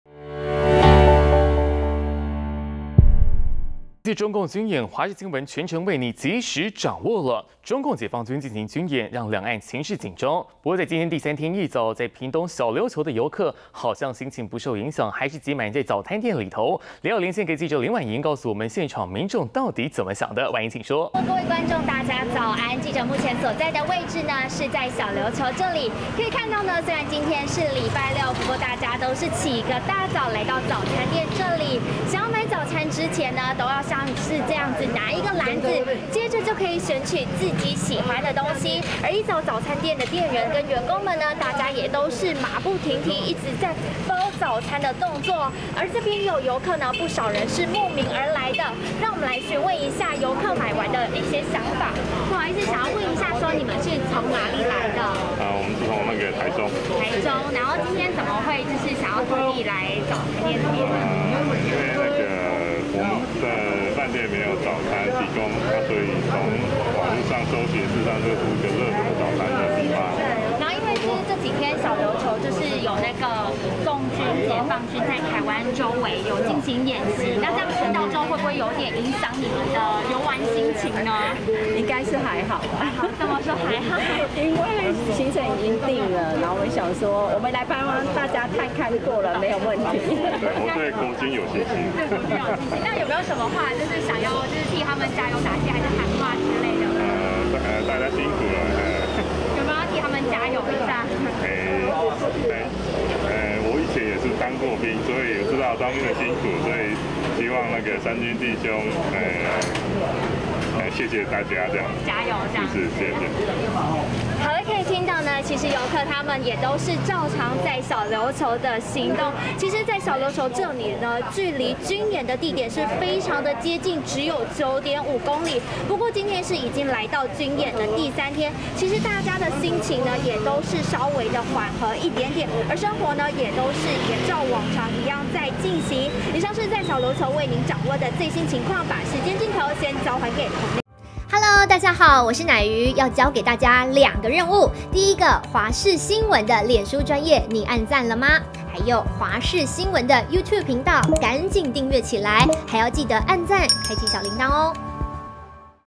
中共軍演第三天 小琉球遊客擠滿早餐店｜華視新聞 20220806
中共解放軍在台灣周邊軍演，造成兩岸情勢緊張，但第三天一早，小琉球的遊客依舊不受影響，擠滿早餐店，詳細情形請連線記者林宛瑩、榮昊北。